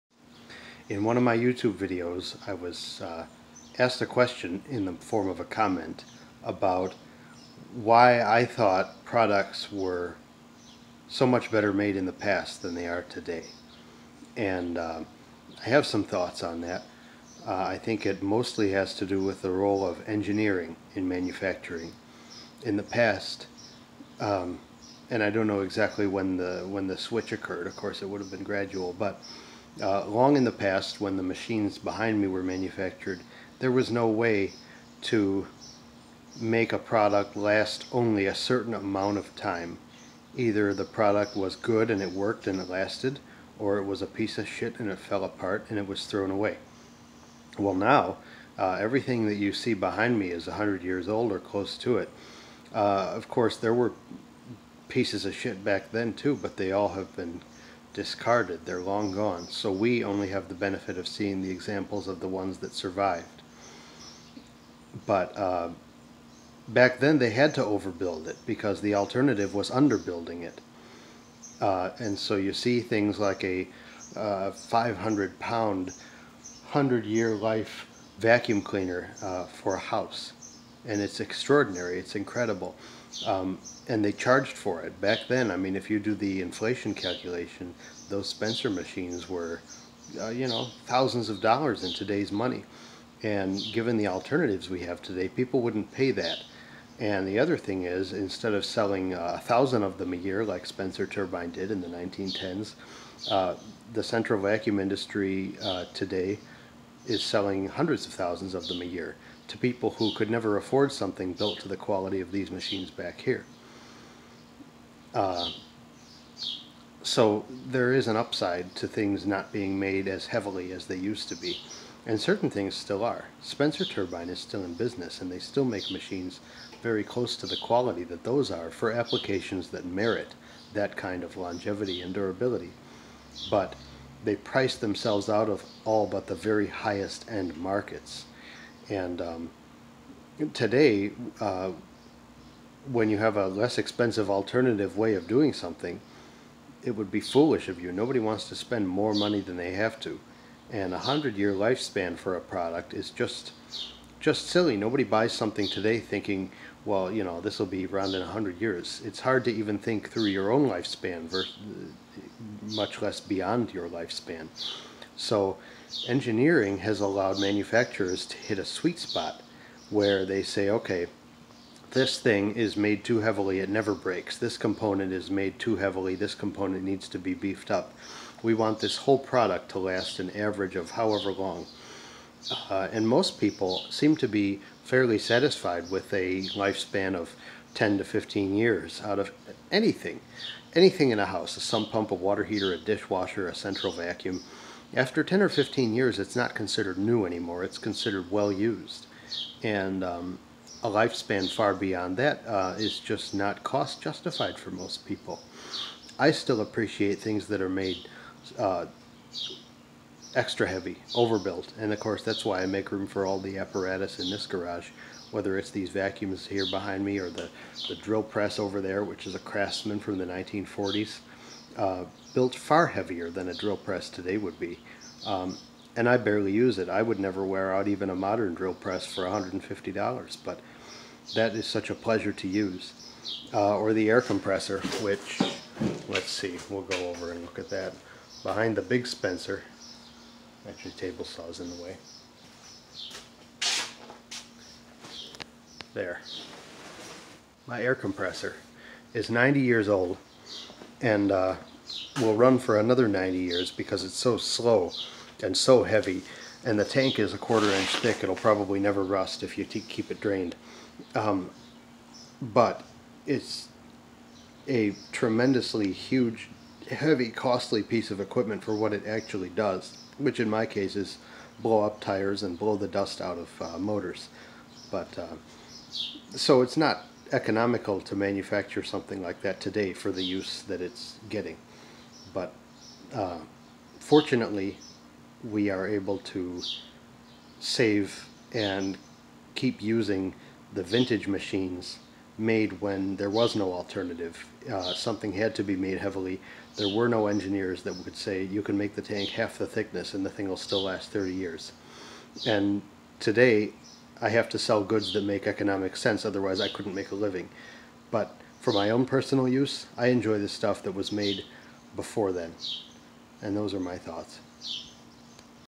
Why was stuff made so much better years ago than it is today? My thoughts.
The role of engineering in product design and life cycles. Designing to last a precise amount of time vs. simply "lasts" or "doesn't last".